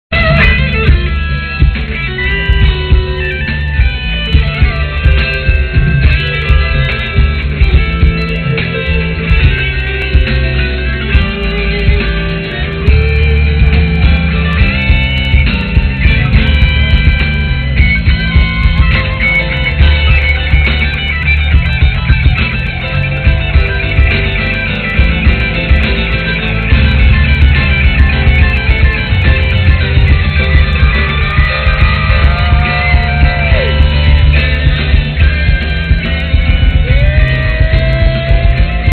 Umphreys at 10KLF
Umphrey's with a little Jake/Brendan guitar duo